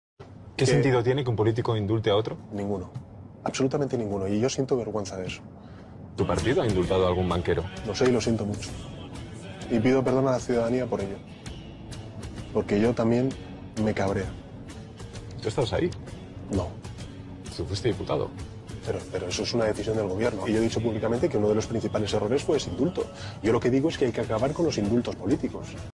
Lo que opinaba Pedro Sánchez de los indultos antes de la condena de Jose Antonio Griñán
El presidente del Gobierno aseguró en una entrevista en 2014: "Siento vergüenza de que un político indulte a otro".

Atrapado uno de los fugitivos más buscados de España